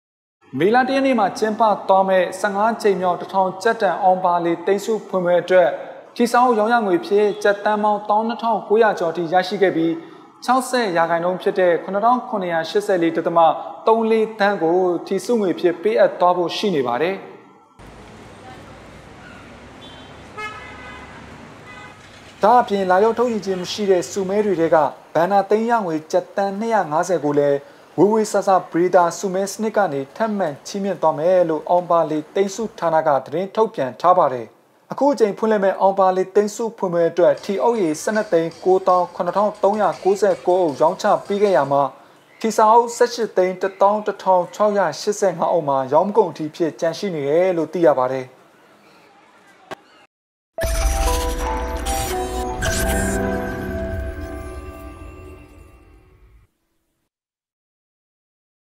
ယနေ့တွင် ဖွင့်လှစ်ခဲ့သည့် အောင်ဘာလေထီဖွင့်ပွဲအတွက် ကျပ် သန်းပေါင်း (၇၀၀၀) ကျော် ချီးမြှင့်မည်
ယနေ့ မေလ (၁) ရက်နေ့တွင် ဖွင့်လှစ်ခဲ့သည့် (၁၅) ကြိမ်မြောက် (၁၀၀၀) ကျပ်တန် အောင်ဘာလေ သိန်းဆုဖွင့်ပွဲအတွက် ထီစာအုပ်ရောင်းချရငွေအဖြစ် ကျပ်သန်းပေါင်း (၁၂၉၀၀) ကျော်ရှိခဲ့ပြီး အဲဒိရဲ့ (၆၀) ရာခိုင်နှုန်းဖြစ်တဲ့ ကျပ်သန်း (၇၀၀၀) ကျော်ကို ထီဆုငွေအဖြစ် ပေးအပ်သွားမှာဖြစ်ပါတယ်။